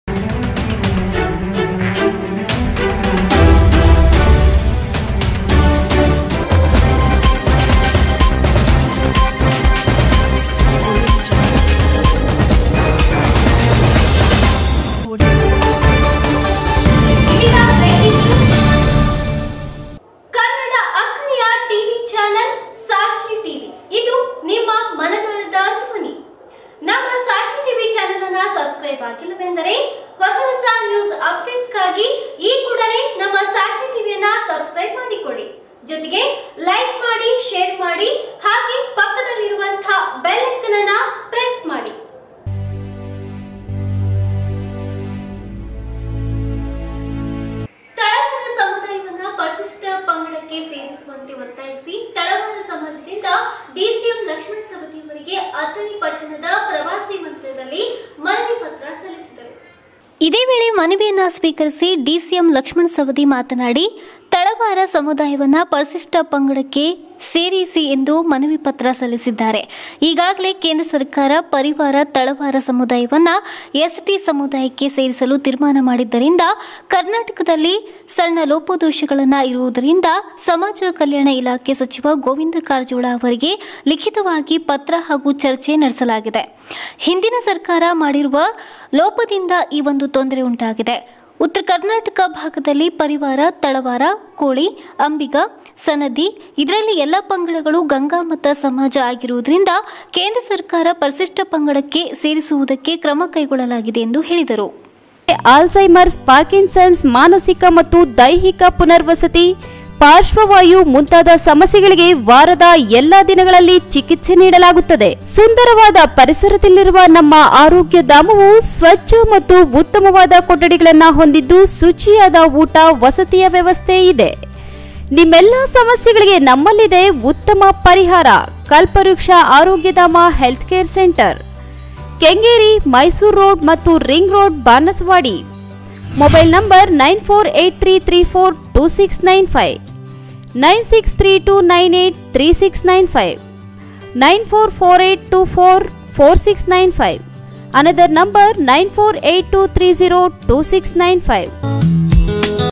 ತಳವಾರ ಸಮುದಾಯಕ್ಕೆ ಎಸ್ ಟಿ ಪ್ರಮಾಣ ಪತ್ರ ನಿಡುವಂತೆ ಡಿಸಿಎಂ ಸವದಿಗೆ ಮನವಿ. |sachi tv
ಅಥಣಿ ವರದಿ

ತಳವಾರ ಸಮುದಾಯಕ್ಕೆ ಎಸ್ ಟಿ ಪ್ರಮಾಣ ಪತ್ರ ನಿಡುವಂತೆ ಡಿಸಿಎಂ ಸವದಿಗೆ ಮನವಿ.

ಅಥಣಿ: ತಳವಾರ ಸಮುದಾಯವನ್ನು ಪರಿಶಿಷ್ಟ ಪಂಗಡಕ್ಕೆ ಸೇರಿ ಎಂದು ಒತ್ತಾಯಿಸಿ ತಳವಾರ ಸಮಾಜದಿಂದ ಡಿಸಿಎಂ ಲಕ್ಷ್ಮಣ್ ಸವದಿ ಅವರಿಗೆ ಅಥಣಿ ಪಟ್ಟಣದ  ಪ್ರವಾಸಿ ಮಂದಿರದಲ್ಲಿ ಮನವಿ ಪತ್ರ ಸಲ್ಲಿಸಿದರು.

ಇದೆ ವೇಳೆ ಮನವಿ ಸ್ವೀಕರಿಸಿ ಡಿಸಿಎಂ ಲಕ್ಷ್ಮಣ್ ಸವದಿ ಮಾತನಾಡಿ, ತಳವಾರ ಸಮುದಾಯವನ್ನು ಪರಿಶಿಷ್ಟ ಪಂಗಡಕ್ಕೆ ಸೇರಿದ ಎಂದು ಮನವಿ ಪತ್ರ ಸಲ್ಲಿಸಿದ್ದಾರೆ, ಈಗಾಗಲೇ ಕೇಂದ್ರ ಸರ್ಕಾರ ಪರಿವಾರ-ತಳವಾರ ಸಮುದಾಯವನ್ನು ಎಸ್ ಟಿ ಸಮುದಾಯಕ್ಕೆ ಸೇರಿಸಲು ತಿರ್ಮಾನ ಮಾಡಿದ್ದರಿಂದ , ಕರ್ನಾಟದಲ್ಲಿ ಸನ್ನ ಲೋಪದೋಷಗಳನ್ನು ಇರುವುದರಿಂದ ಸಮಾಜ ಕಲ್ಯಾಣ ಇಲಾಖೆ ಸಚಿವ ಗೋವಿಂದ ಕಾರಜೋಳ ಅವರಿಗೆ ಲಿಖಿತವಾಗಿ ಪತ್ರ ಹಾಗೂ ಚರ್ಚೆ ನಡೆಸಲಾಗಿದೆ.
ಹಿಂದಿನ ಸರ್ಕಾರ ಮಾಡಿರುವ ಲೋಪದಿಂದ ಈ ತೊಂದರೆ ಉಂಟಾಗಿದೆ. ಉತ್ತರ ಕರ್ನಾಟಕ ಭಾಗದಲ್ಲಿ ಪರಿವಾರ, ತಳವಾರ, ಕೊಳಿ, ಅಂಬಿ, ಸನದಿ, ಇದರಲ್ಲಿ ಎಲ್ಲ ಪಂಗಡಗಳು ಗಂಗಾಮತ ಸಮಾಜ ಆಗಿರುವುದರಿಂದ ಕೇಂದ್ರ ಸರ್ಕಾರ ಪರಿಶಿಷ್ಟ ಪಂಗಡಕ್ಕೆ ಸೇರಿಸುವದಕ್ಕೆ ಕ್ರಮ ಕೈಗೊಳ್ಳಲಾಗಿದೆ ಎಂದು ಹೇಳಿದರು.

ಹಿಂದಿನ ಸರ್ಕಾರ ಮಾಡಿರುವ ಲೋಪವನ್ನು ಹಾಗೂ ಈಗಿನ ತೊಂದರೆ ಬಗ್ಗೆ ಸಮಾಜ ಕಲ್ಯಾಣ ಸಚಿವ ಗೋವಿಂದ ಕಾರಜೋಳ ನನಗೆ ತಿಳಿಸಿದ್ದಾರೆ, ರಾಜ್ಯ ಸರ್ಕಾರದಿಂದ ಕೆಲವು ತಿದ್ದುಪಡಿ ಮಾಡಿ ಈಗಾಗಲೇ ಕೇಂದ್ರ ಸರ್ಕಾರಕ್ಕೆ ಶಿಫಾರಸು ಮಾಡಲಾಗಿದೆ, ತಳವಾರ ಸಮುದಾಯದವರು ಯಾವುದೇ ಆತಂಕಕೆ
 ಒಳಗಾಗಬೇಡಿ, ನಿಮ್ಮ ಪರವಾಗಿ ದೇಶದ ಪ್ರಧಾನಿ ನರೇಂದ್ರ ಮೋದಿ ಅವರು ತಿರ್ಮಾನ ಮಾಡಿದ್ದಾರೆ, ಕೇಂದ್ರ ಸರ್ಕಾರದಲ್ಲಿ ಯಾವುದೇ ತಿರ್ಮಾನವನ್ನು ರಾಜ್ಯ ಸರ್ಕಾರ ಯಥಾವತ್ತಾಗಿ ಜಾರಿಗೆ ತರಲಾಗುವುದು ಎಂದು ಭರವಸೆ ನೀಡಿದರು.

ಬೈಟ್_ ಡಿಸಿಎಂ ಲಕ್ಷ್ಮಣ್ ಸವದಿ.

ವರದಿ ಡಾ ಆರ್ ಎಸ್ ದೊಡ್ಡನಿಂಗಪ್ಪ ಗೋಳ  ಅಥಣಿ